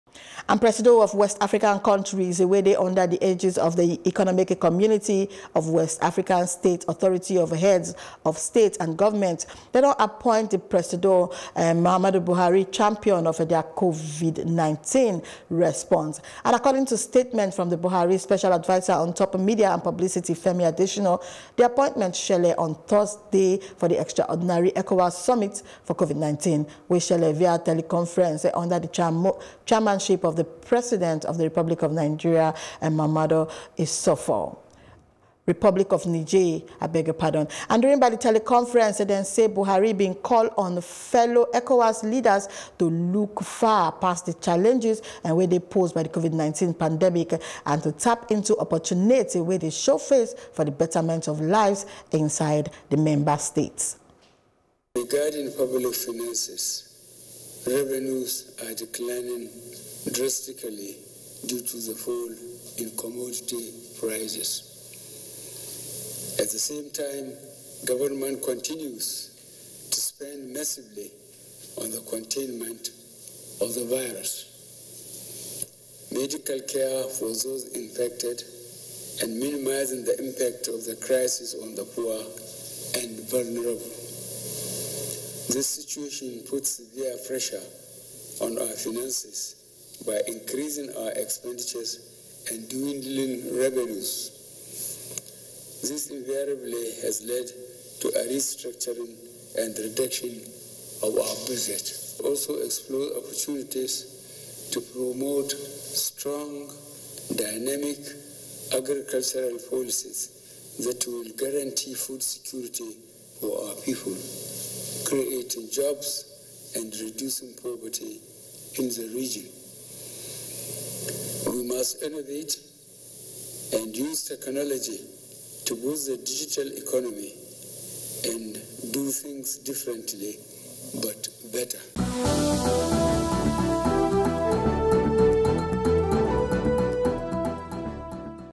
ECOWAS Leader Don Appoint Presido Buhari Oga Of COVID-19
Presido of West African countries, wey dey under the aegis of the Economic Community of West African States Authority of Heads of State and Government, don appointed the President Muhammadu Buhari Champion of their COVID-19 response.